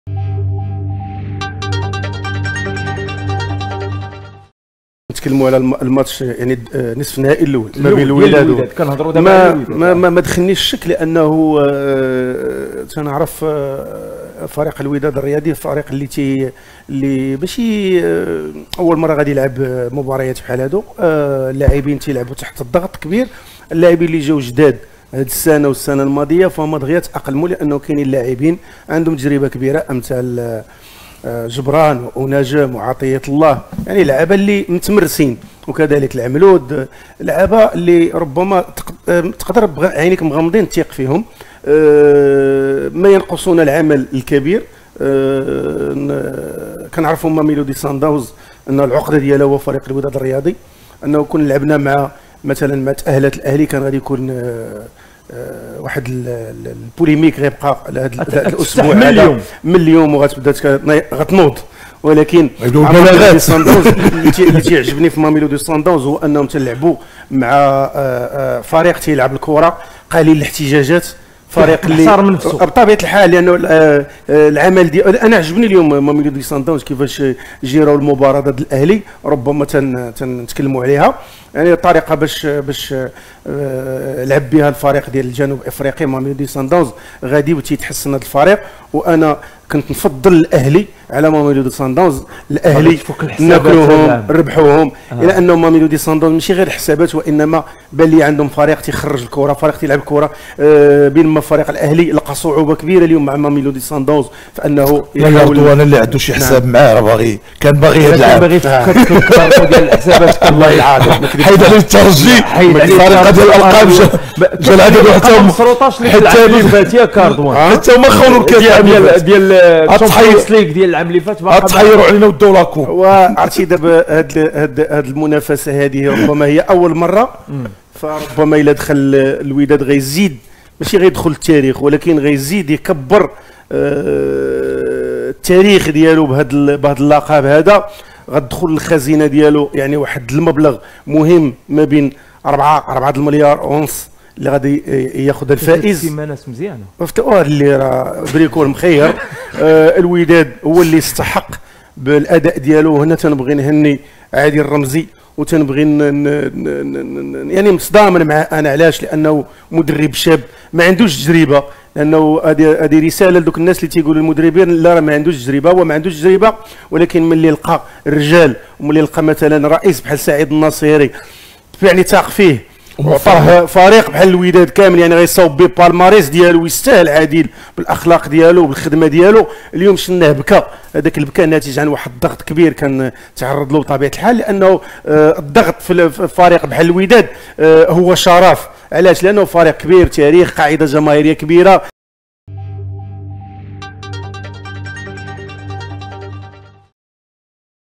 رضوان العلالي: صان داونز كيلعبو الكرة وقليل الاحتجاجات وكتبقى الوداد عقدة لهم
تحدث اللاعب السابق لنادي الوداد الرياضي رضوان العلالي عن مواجهة الوداد وماميلودي صان داونز